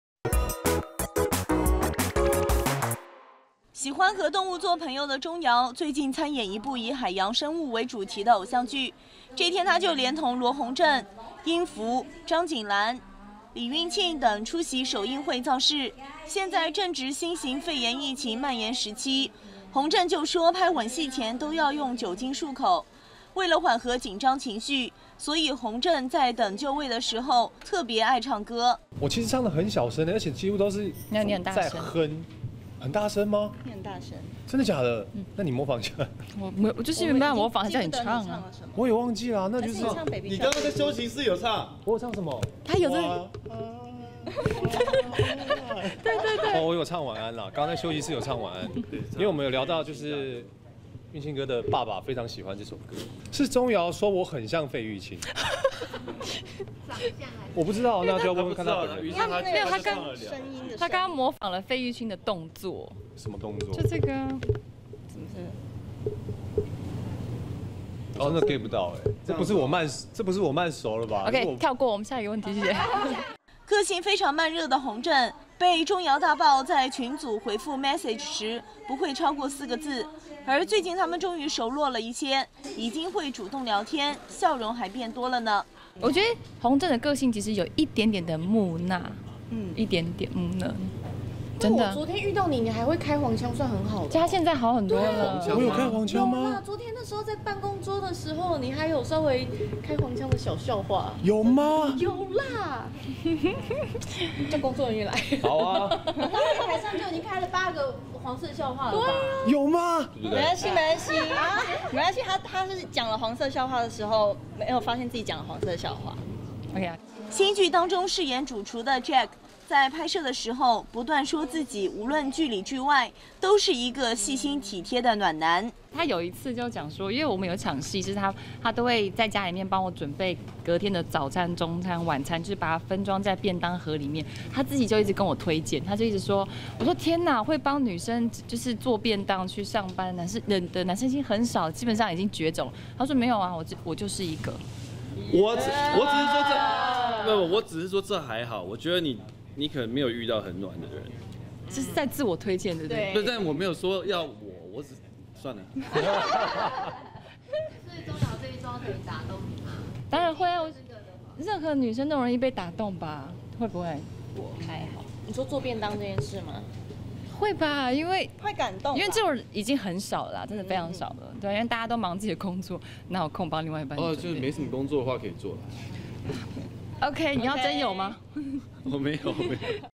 羅宏正片場常哼歌 被鍾瑤讚像費玉清
羅宏正與鍾瑤等人偶像劇首映會，愛在片場哼歌，被鍾瑤讚聲線像費玉清，又被爆經常開黃腔